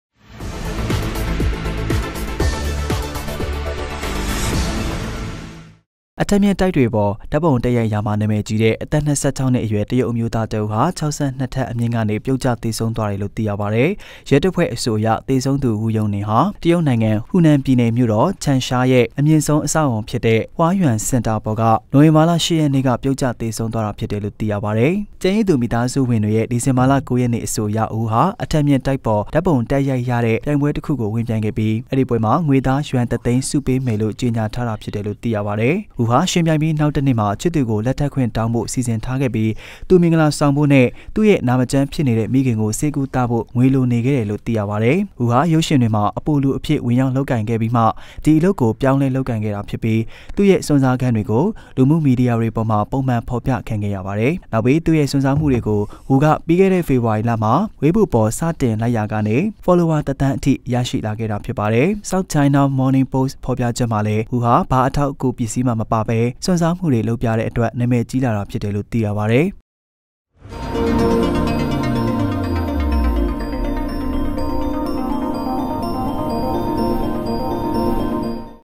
အထပ္ျမင္႔တိုက္မ်ားေပၚ ဓာတ္ပံံုတက္ရိုက္ရာတြင္ နာမည္ႀကီးသည့္ တရုတ္အမ်ဳိးသားတစ္ဦး
အထပ္ျမင္႔တိုက္ေတြေပၚဓာတ္ပံုတက္ရိုက္ရာမွာနာမည္ႀကီးတဲ႔ အသက္ ၂၆ ႏွစ္အရြယ္တရုတ္အမ်ဳိးသားတစ္ဦး ၆၂ ထပ္အျမင္႔ကေန ျပဳတ္က်ေသဆံုးသြားတယ္လို႔ သိရပါတယ္။